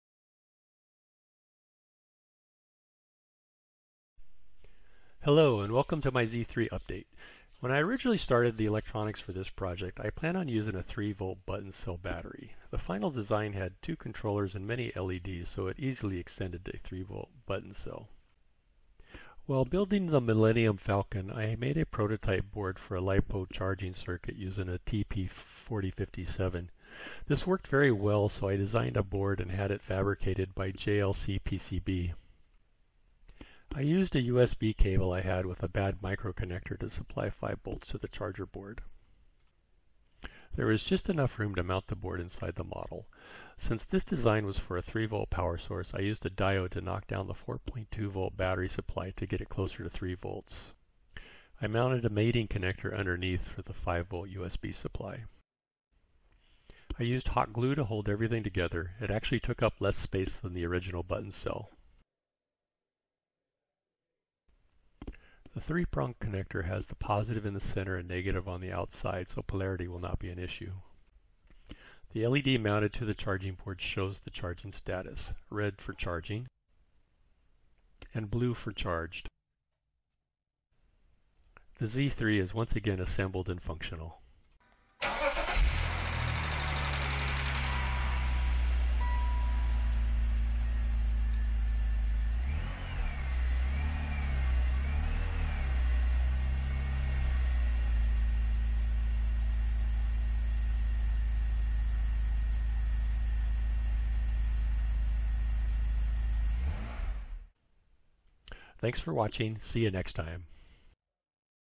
BMW Z3 Updated
My BMW Z3's - Tamiya BMW Z3 Roadster kit, Battery update
This is an update to my build of the BMW Z3 Roadster by Tamiya where I swicth to a LiPo battery and charger circuit.

Electronics:
TP4057 LiPo Charger Circuit